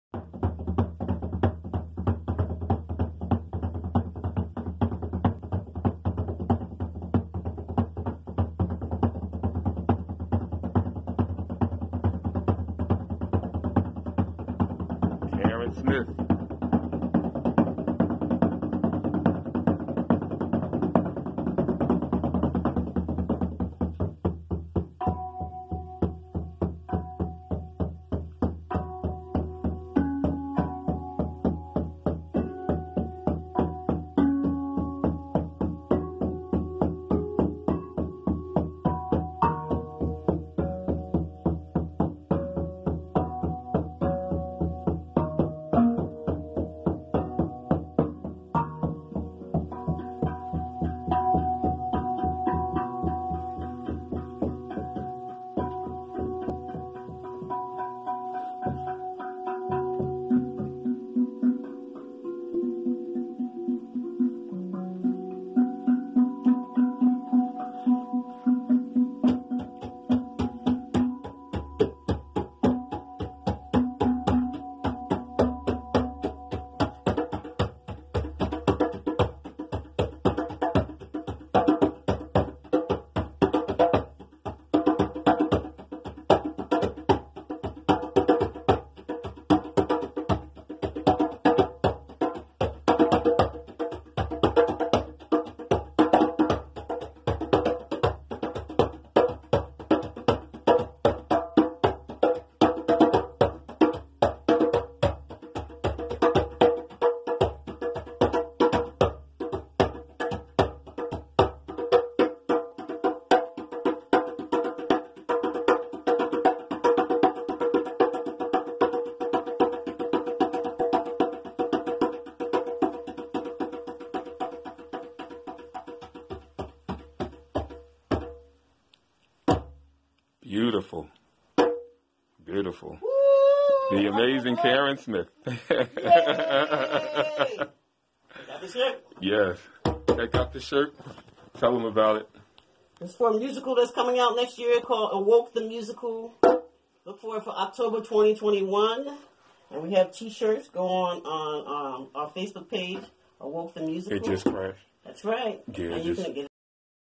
Rehearsal for Poet Demetrius Ford 's Negritude Recording
Creating the background for OMAZING new works by Poet Demetrius Ford.  With Samori Coles engineering.